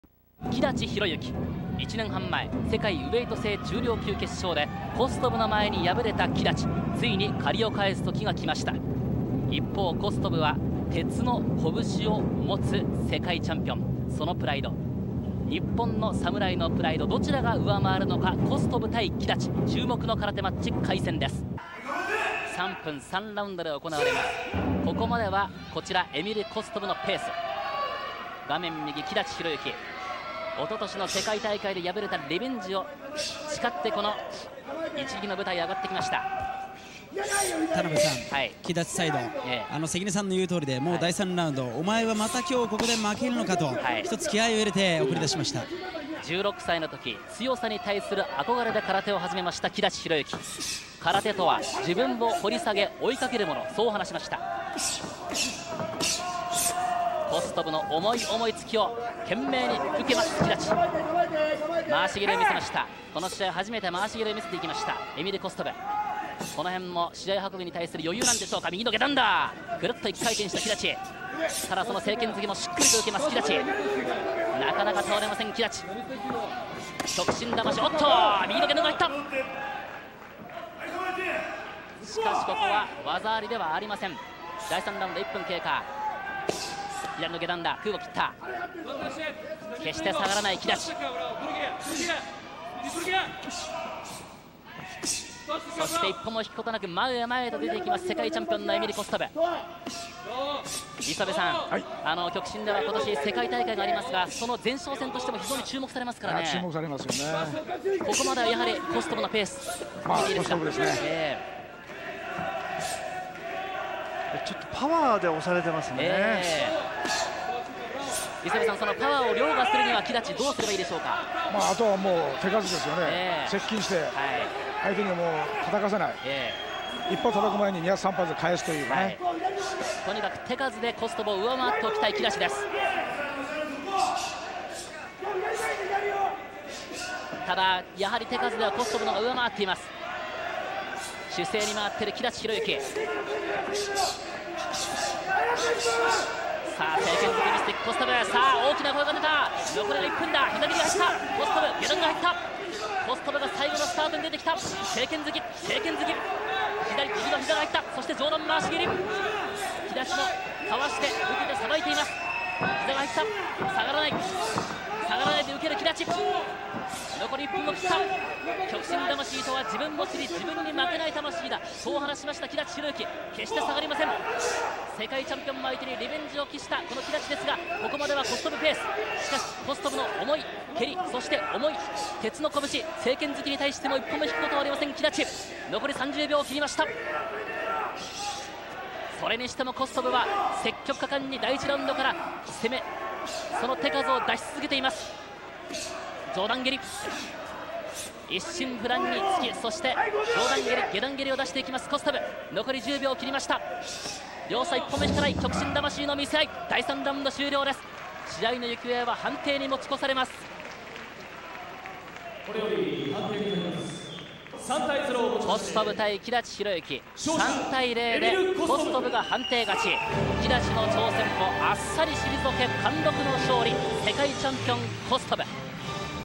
Emil Kostov Vs. Hiroyuki Kidachi (22/02/2003)
Emil Kostov Vs. Hiroyuki Kidachi
K-1 Ichigeki Tokyo 2003
February 22, 2003 | Tokyo, Japan

Contest with kyokushin rules.